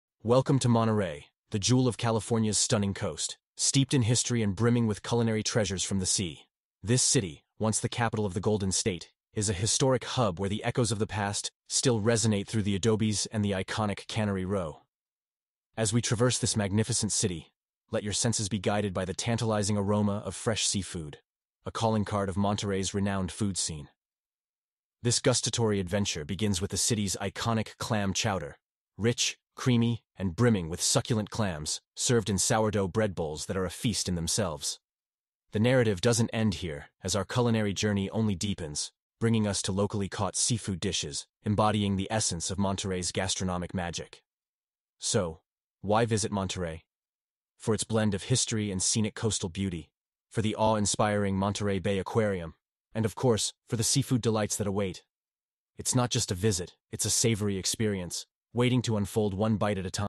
🌊 Monterey, California | Coastal Wonders!
🌊✨ Dive into the rich history and breathtaking beauty of Monterey, California! 🏛️🌅 Discover the state's first capital, wander through historic Cannery Row, and step back in time with the charming adobes. 🏠🍃 Savor the freshest seafood - from clam chowder in sourdough bowls to locally caught fish. 🐟🥖 Don't miss the world-renowned Monterey Bay Aquarium and the stunning coastal views! 🐠📸

00:00:00 welcome to montere the jewel of California's stunning Coast steeped in history and brimming with culinary Treasures from the sea this city once the capital of the golden state is a historic Hub where the Echoes of the past still resonate through the Adobes and the iconic Canary row as we Traverse This Magnificent City let your senses be guided by the tantalizing Aroma of fresh seafood a calling card of Mon's renowned food scene this gustatory Adventure begins with the city's iconic clam chowder Rich creamy and brimming with

00:00:31 succulent clams served in sourdough bread bowls that are a feast in themselves The Narrative doesn't end here as our culinary Journey Only deepens bringing us to locally caught seafood dishes embodying the essence of Mon's gastronomic magic so why visit Monteray for its blend of history and Scenic Coastal Beauty for the awe inspiring Monteray Bay Aquarium and of course for the seafood Delights that await it's not just a visit it's a Savory experience waiting to unfold one bite at a time